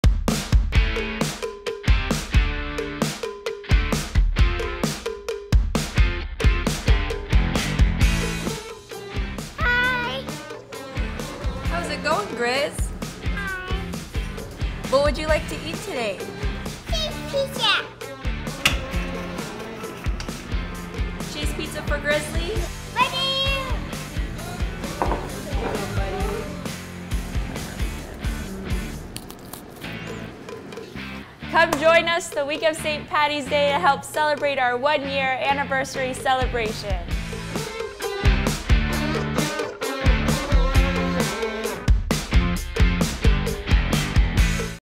Allie's Pizza (Promo Spot) - Shot on URSA Mini
Great vegan food, great atmosphere!  I had the opportunity to shoot this and it was my first real project with the URSA Mini.